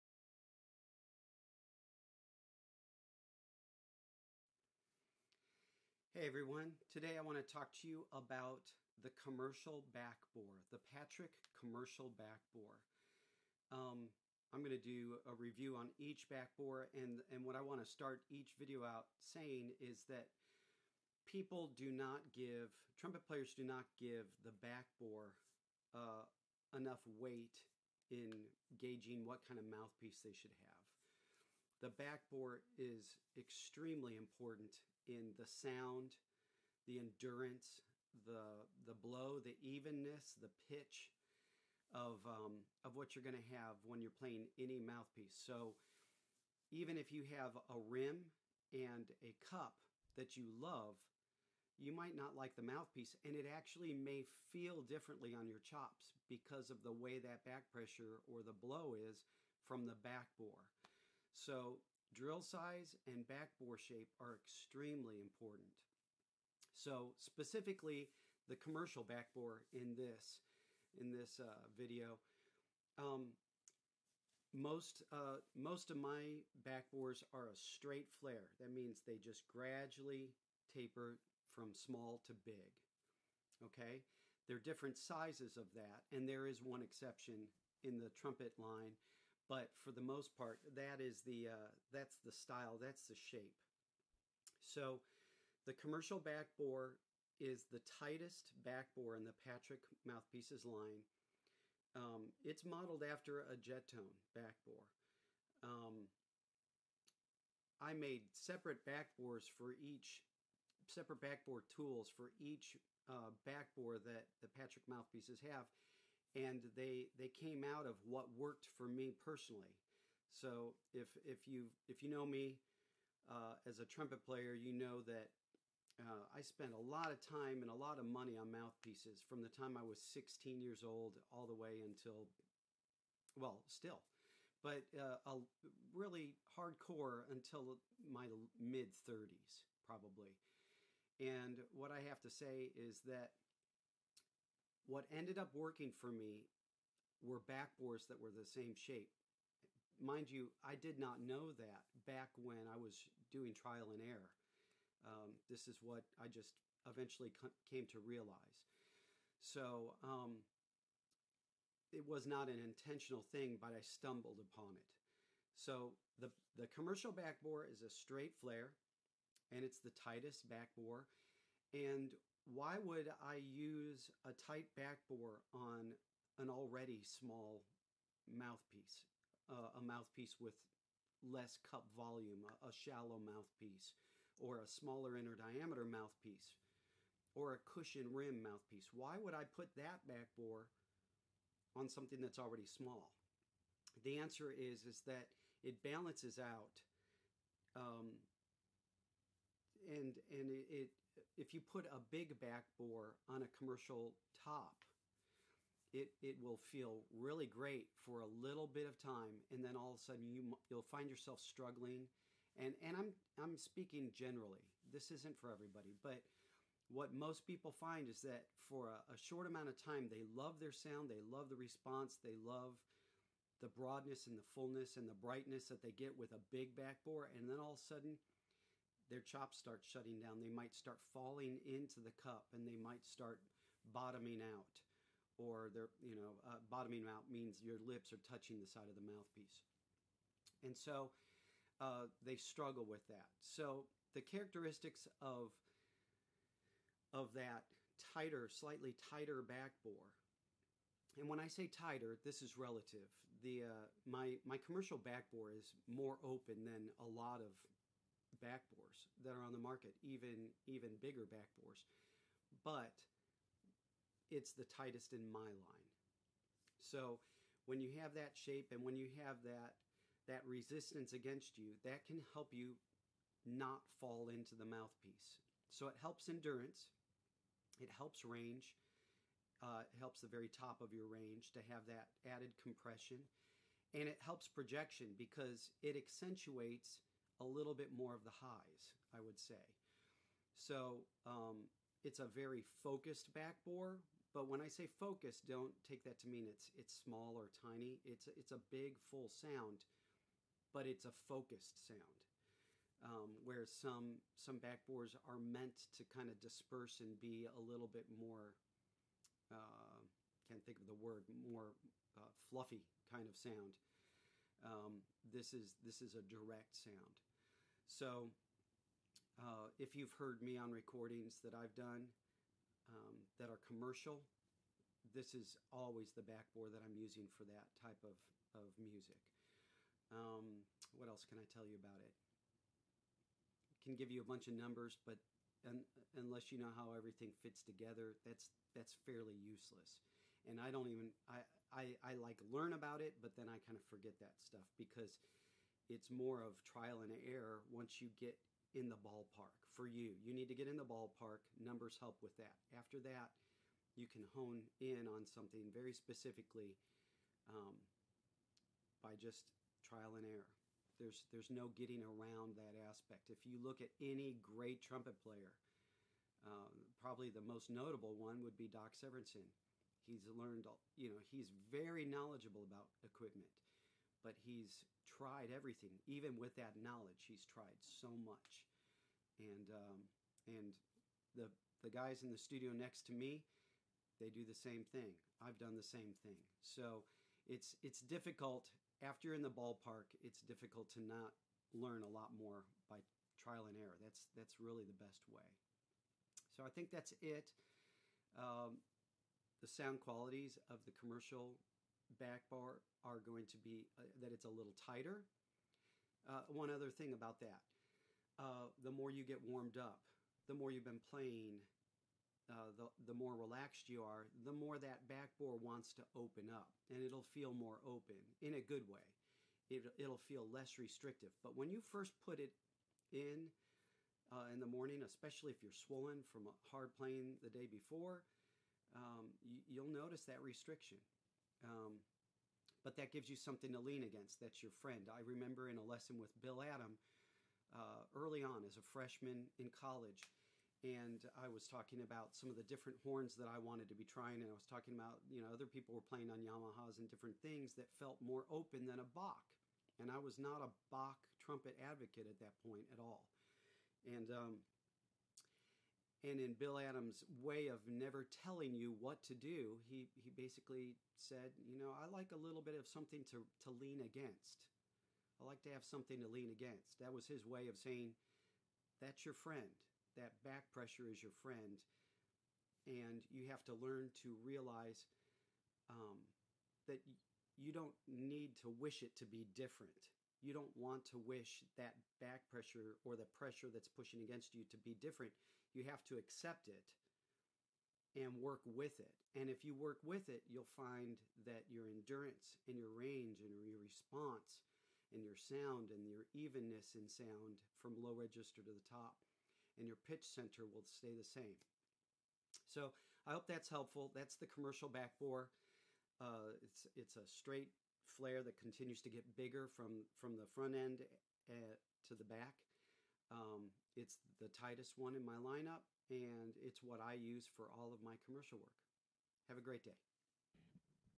Commercal Back-bore Review - PATRICK Mouthpieces
Our most popular back-bore, the commercial back-bore is designed to pair perfectly with our commercial tops to help with compression that does not "back up" on the player.